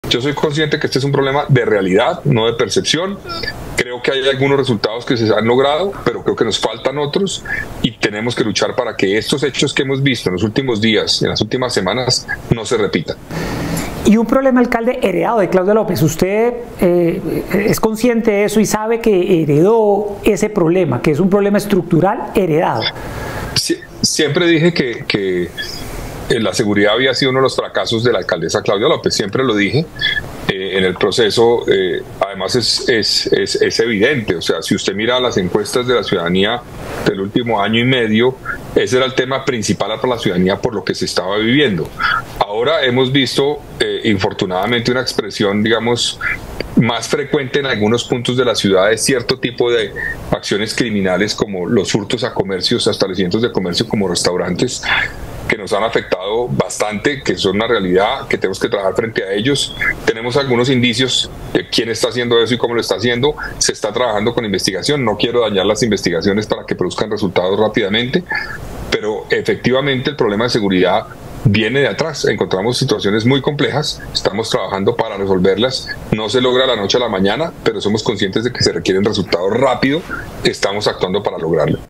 “La seguridad fue uno de los fracasos de Claudia López" | Semana Noticias
"Siempre lo dije, la seguridad fue uno de los fracasos de la alcaldesa Claudia López (...) Efectivamente el problema de seguridad viene de atrás”: Carlos Fernando Galán sobre cómo recibió a Bogotá.